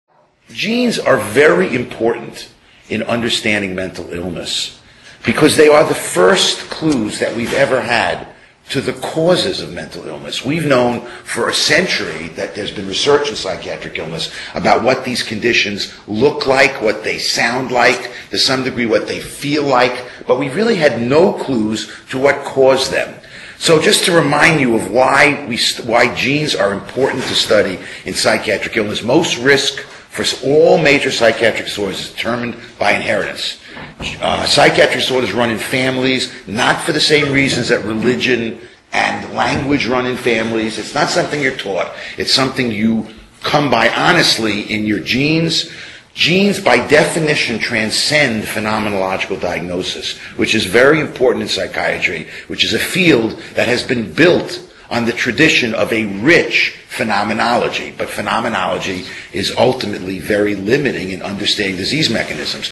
Why Genes are Important to Mental Illness
The National Institute of Health's  Daniel Weinberger, MD, explains how genes are the first clues we have ever had as to the causes of mental illness.

Watch Dr. Weinberger's full Grand Rounds from the University of Florida Department of Psychiatry